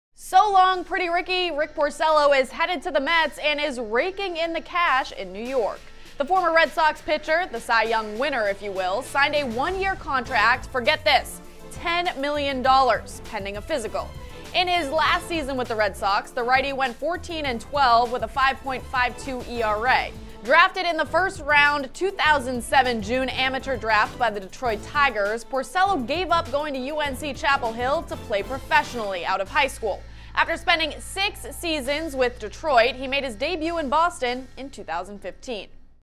Rick Porcello Signs New Deal With New York Mets
Former Boston Red Sox pitcher Rick Porcello has signed a new deal with the New York Mets. NESN's Courtney Cox details the contract and his time at Fenway Park.